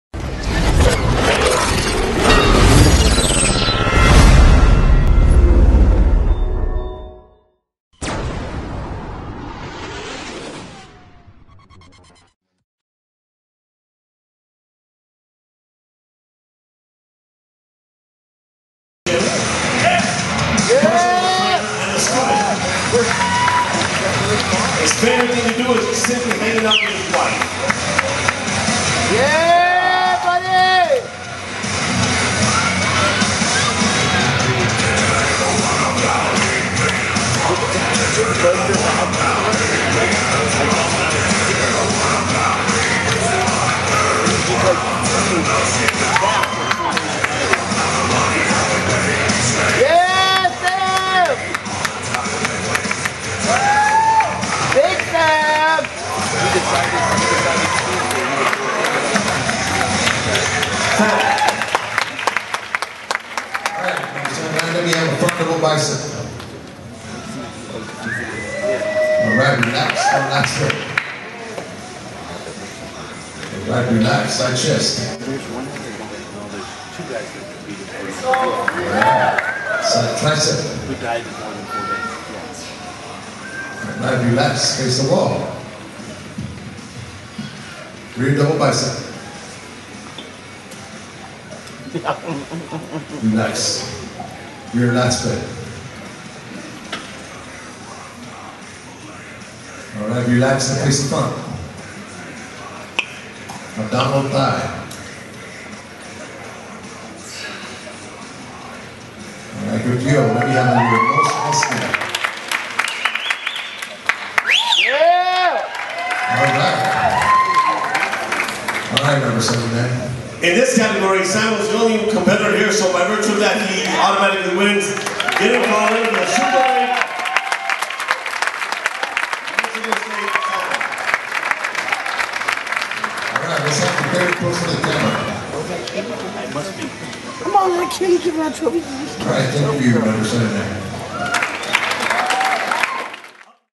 Sam Schrader - NM/Mid USA Bodybuilding 2013 - Superheavyweight Winner
Sam Schrader wins the 2013 NM/Mid USA Bodybuilding Superheavyweight class. July 6, 2013. Look for a special appearance by Katie Schrader, his wife. :-)